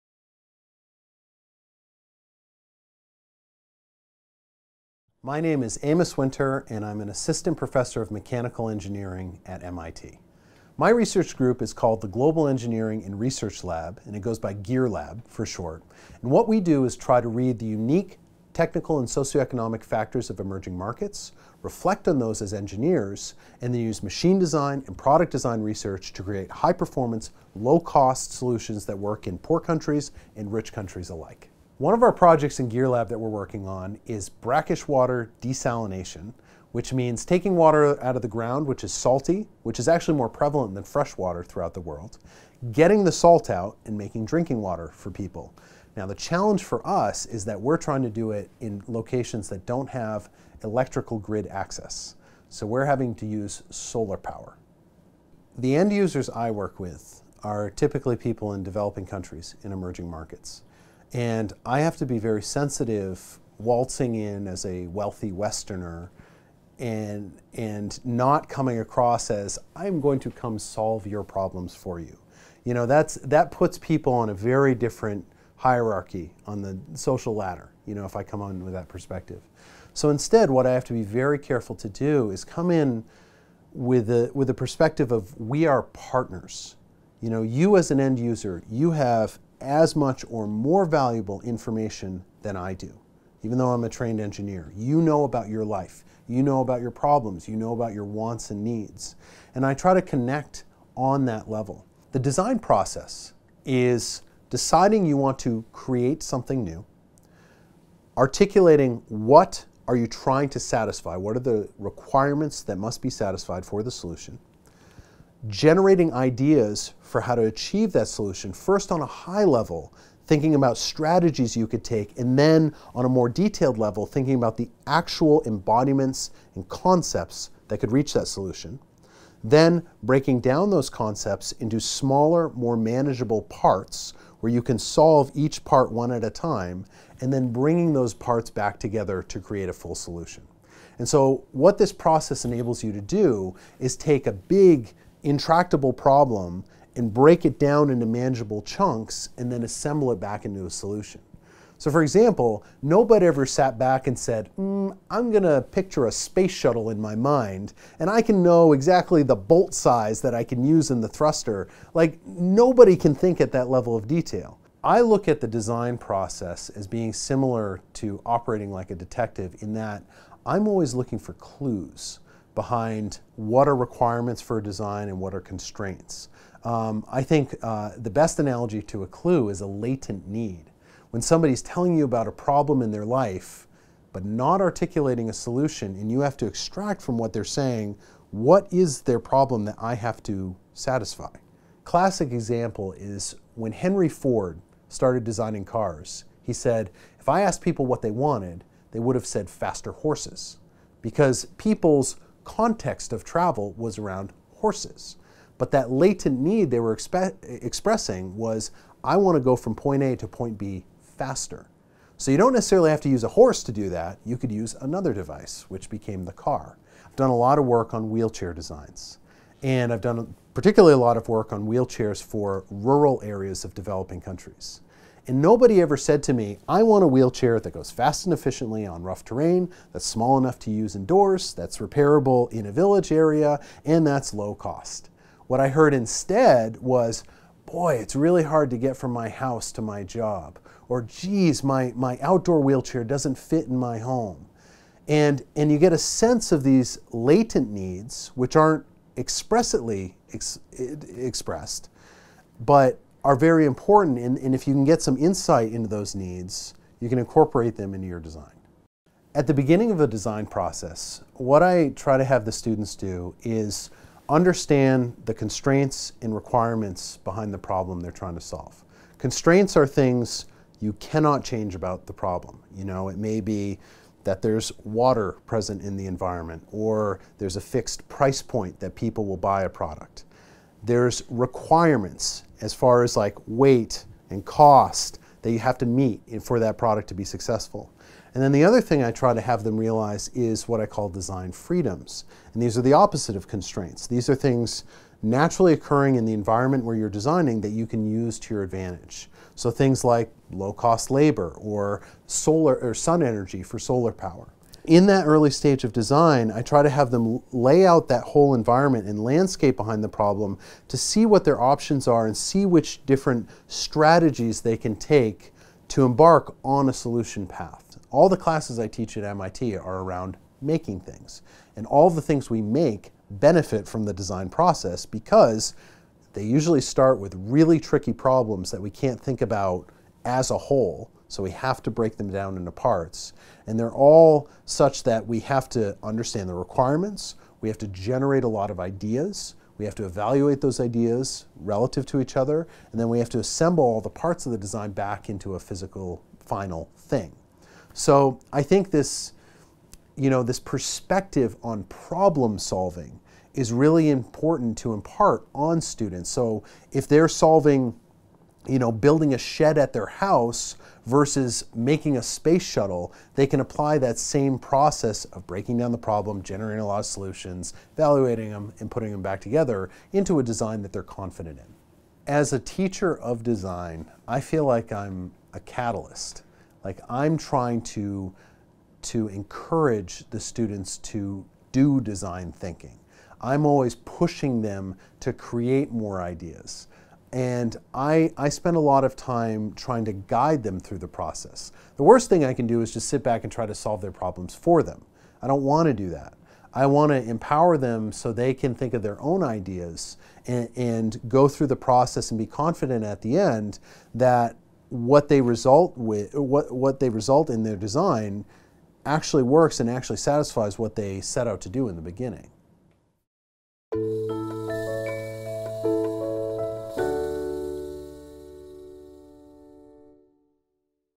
Amos Winter: Connecting with Users
Amos Winter, Assistant Professor in the Department of Mechanical Engineering at MIT, introduces us to his research group, the Global Engineering and Research (GEAR) Lab. He describes their work in developing countries (0:32). He then gives his thoughts on why using the design process is like being a detective (1:43) and his approach to teaching design to students at MIT (4:20).

This is a video segment from the Teaching Systems Lab's massive open online course "Design Thinking for Leading and Learning".